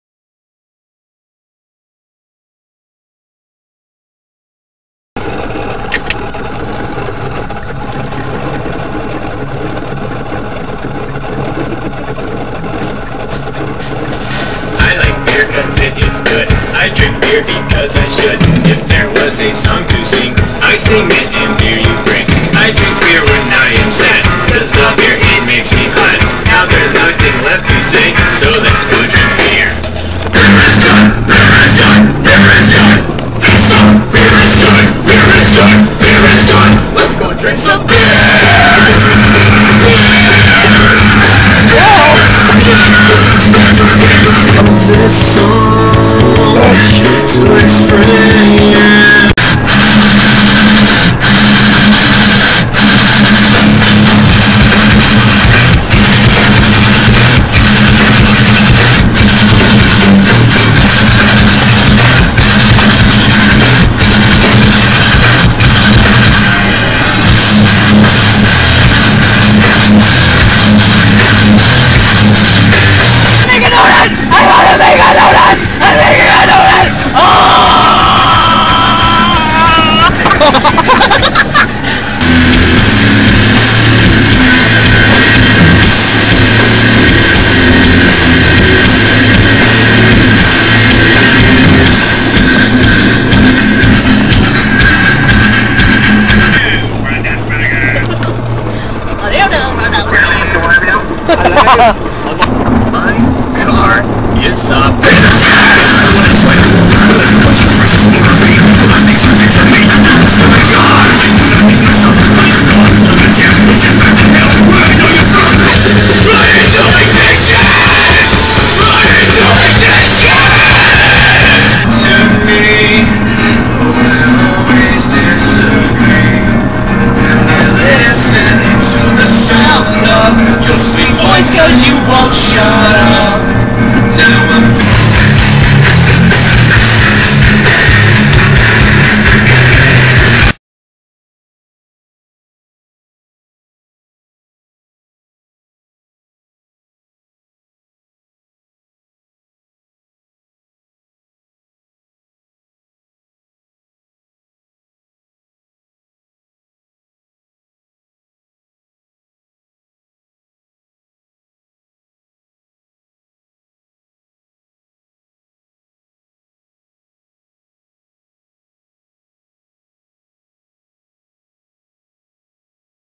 Ghost and Ogletree -We Love Psychostick
These are some clips from a car ride to the movies
and no we arent hamming it up for the camera--this is how we really are--arf